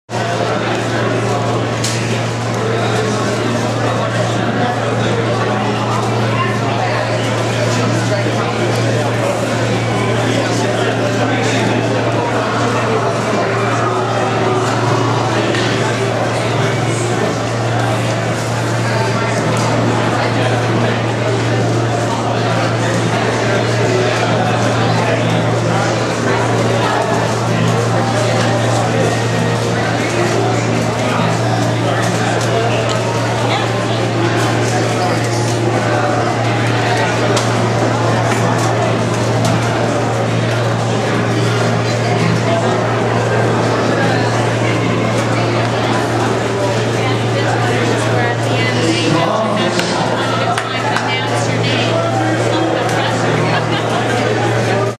Phil Thomas solve 1 - 16.40s - US Nationals 2008 final
Phil Thomas solve 1 of 5 (16.40 seconds) during US Nationals and Open 2008 final at Underground Atlanta.